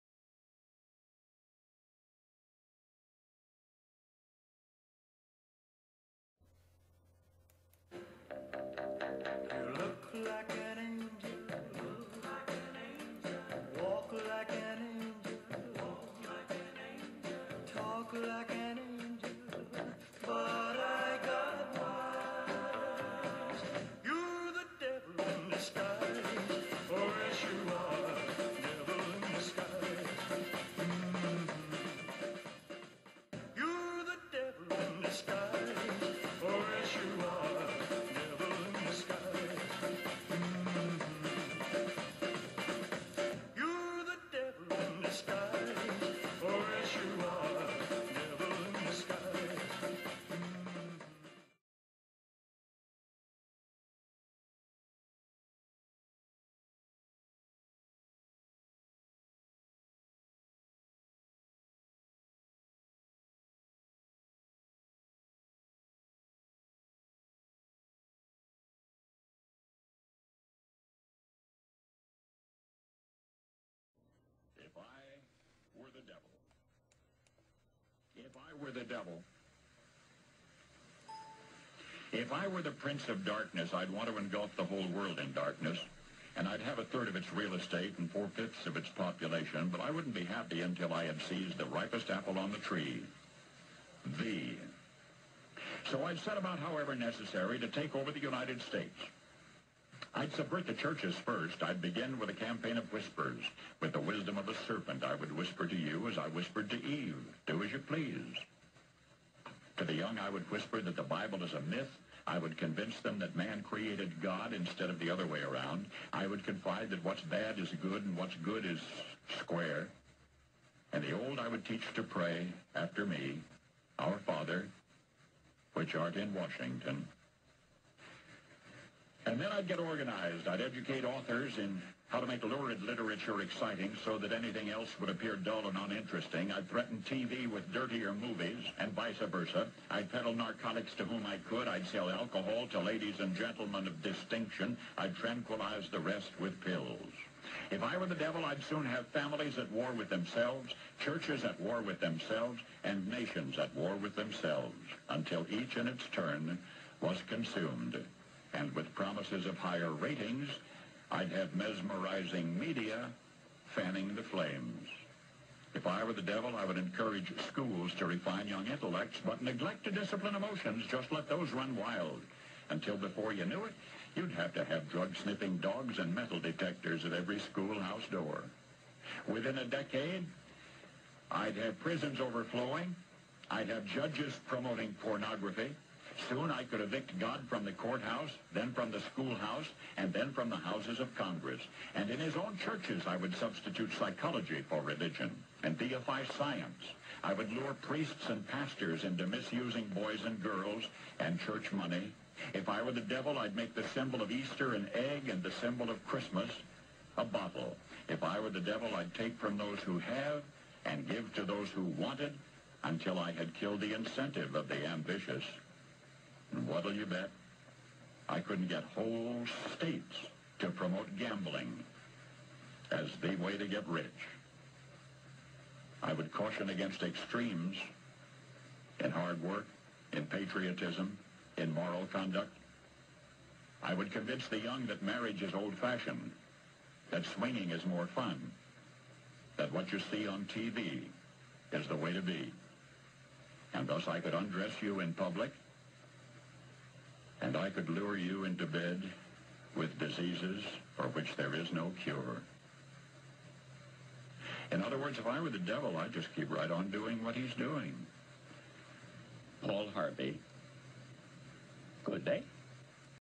When we Vote, We Support This!
I had heard of the use of depleted uranium munitions by Western "democracies" in the Middle East, but it was not until I stumbled upon these photos, that I realised what terrible crimes have been committed in our name.  When you pay taxes, you buy into this.  When you vote, you buy into this.  This is not the time to remain silent.  I also include a speech by Paul Harvey, which was prophetic and very interesting.  I have never considered myself to be religious, but I am beginning to wonder whether the establishment may need to fear the prospect of hell.